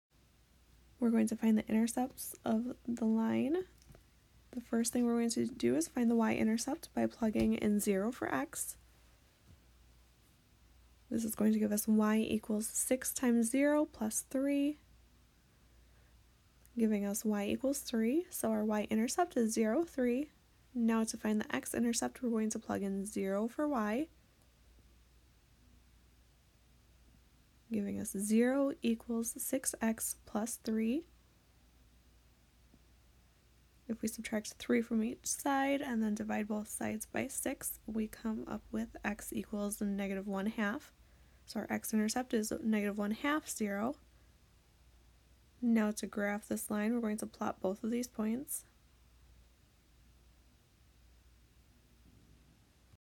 Line: Find the intercepts of the line y =6x + 3
Tiala from SVSU Micro Math helps you find the x-intercept and y-intercept of a line.

Problem: Find the intercepts of the line y =6x + 3